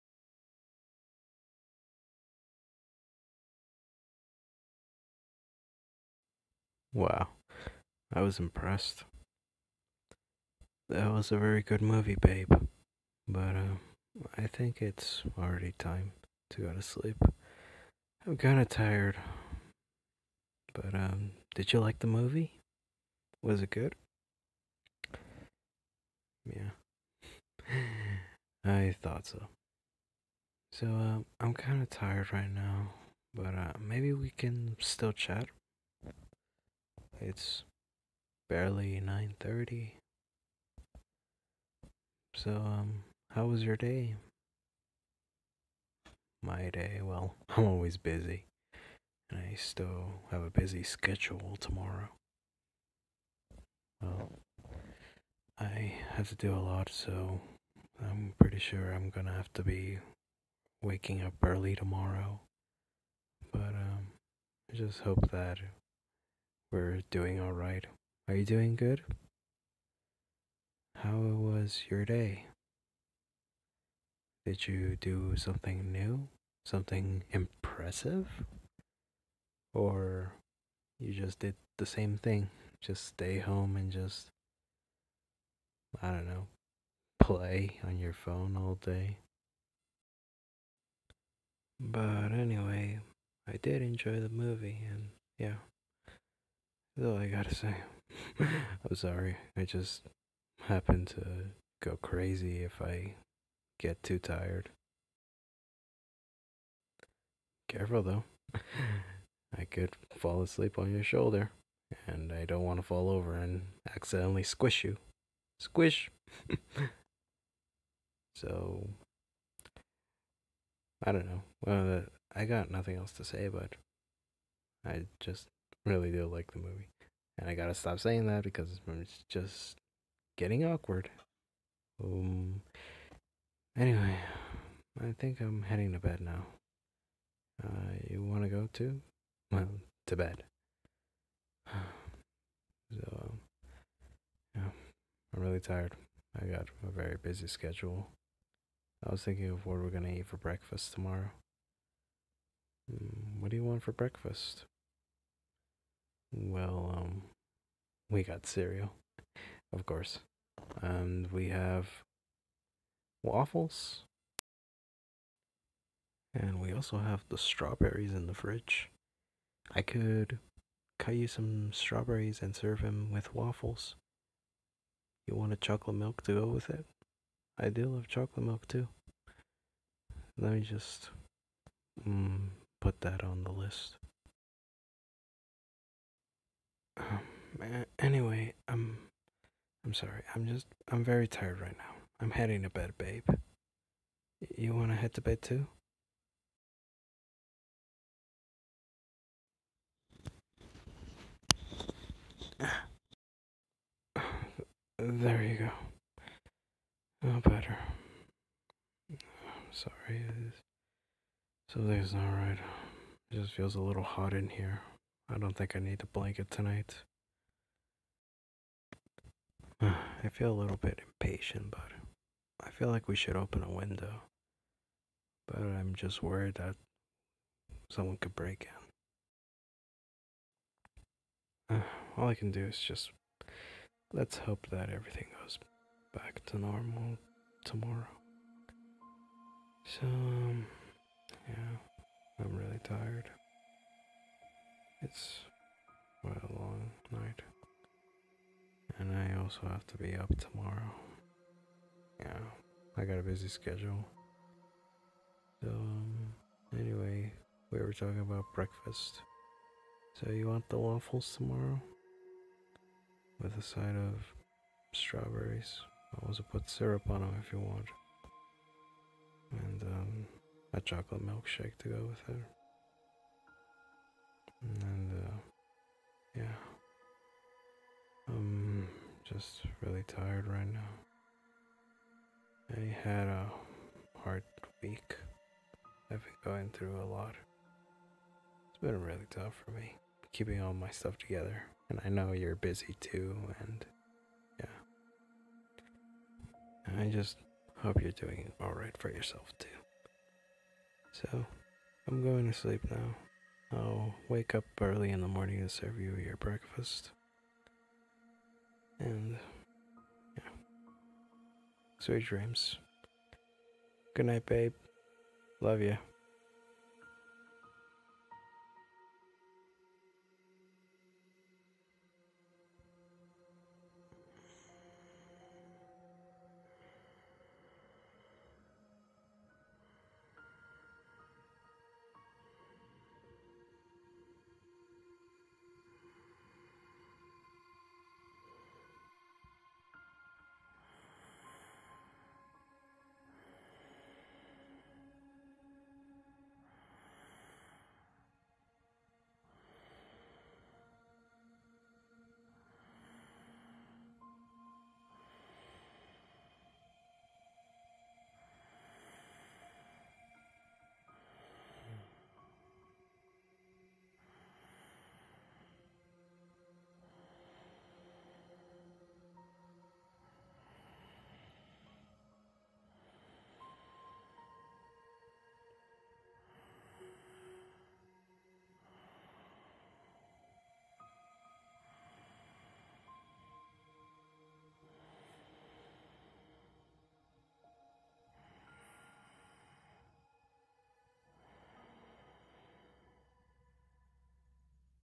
ASMR Roleplay - Boyfriend getting ready for bed 😴
I have never attempted this roleplay so I decided to made it just plain and simple, enjoy.